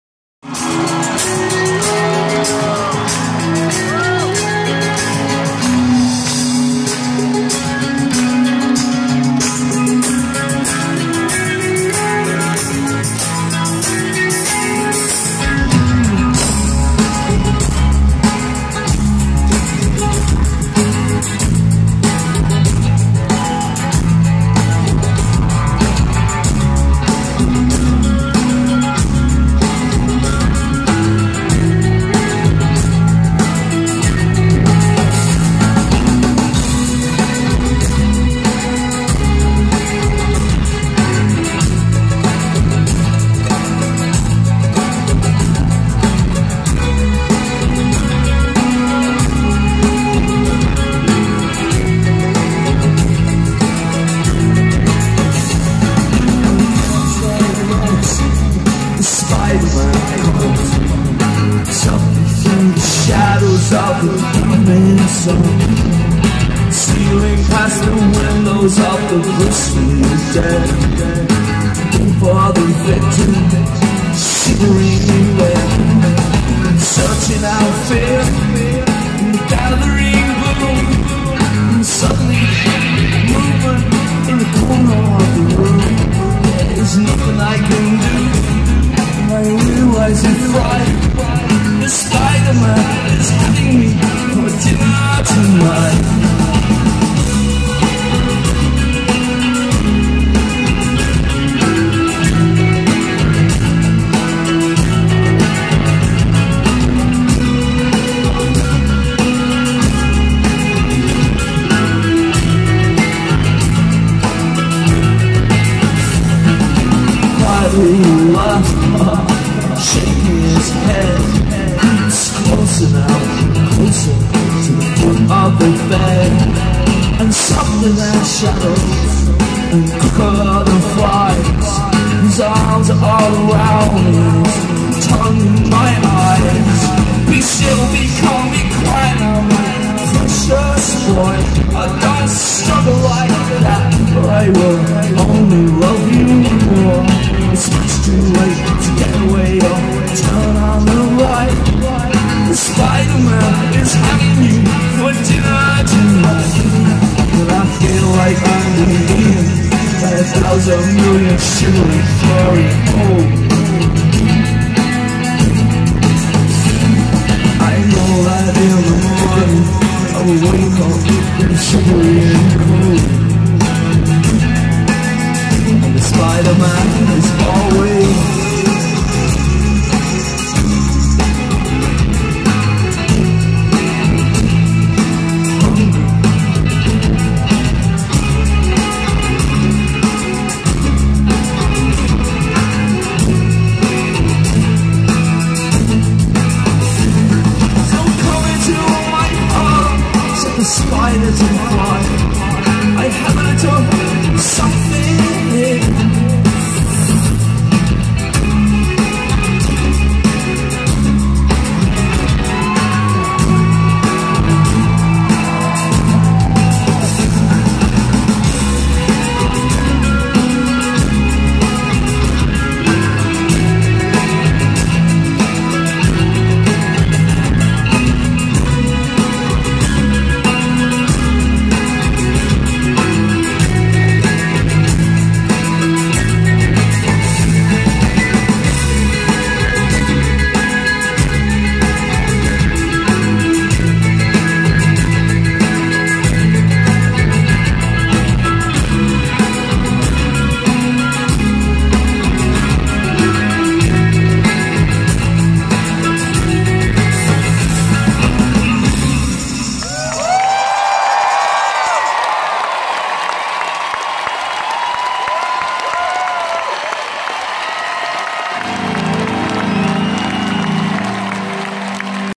The Cure @ Atlas Arena 10 2016
The Cure live performance at Atlas Arena in Łódź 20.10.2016